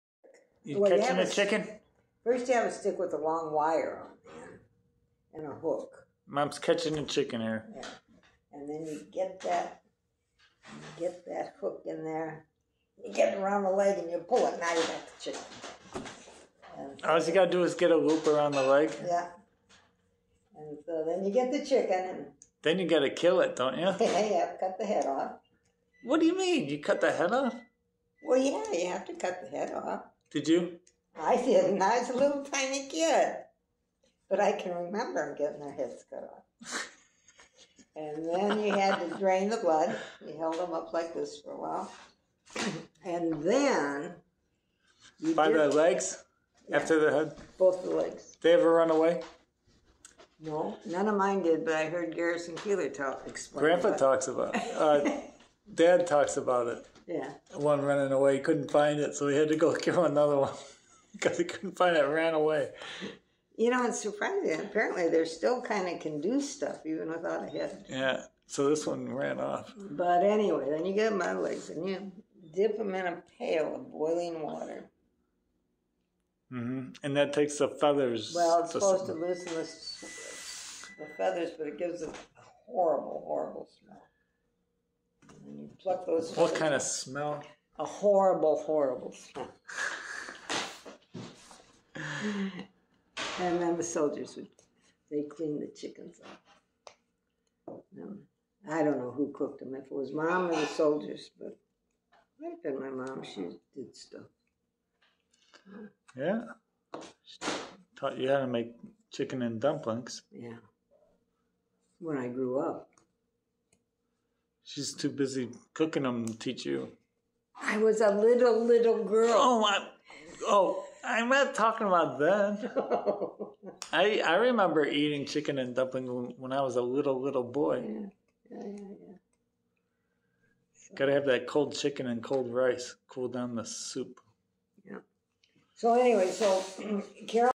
Mom remembers catching a chicken, killing it, and cleaning.
Soldiers had plane trouble and landed and stayed at my mom's farmhouse when she was young.